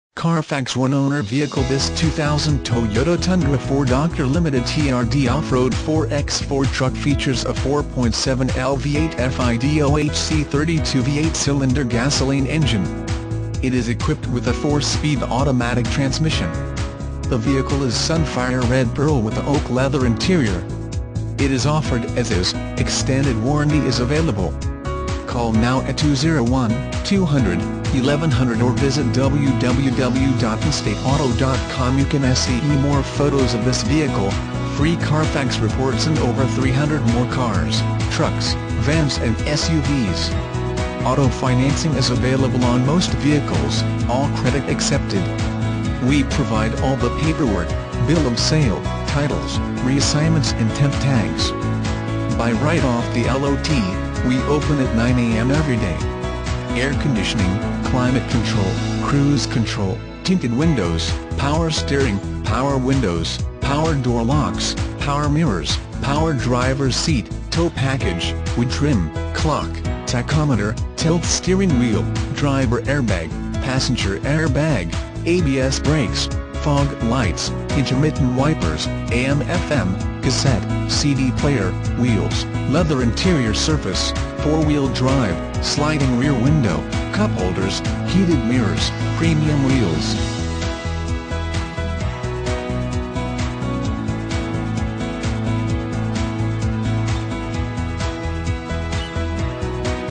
2000 TOYOTA TUNDRA Jersey City, NJ
2000 TOYOTA TUNDRA Jersey City, NJ

-CARFAX 1-OWNER VEHICLE- - This 2000 Toyota Tundra 4dr Limited TRD OFFROAD 4x4 Truck features a 4.7L V8 FI DOHC 32V 8cyl Gasoline engine. It is equipped with a 4 speed automatic transmission. The vehicle is Sunfire Red Pearl with a Oak Leather interior. It is offered As-Is, extended warranty is available.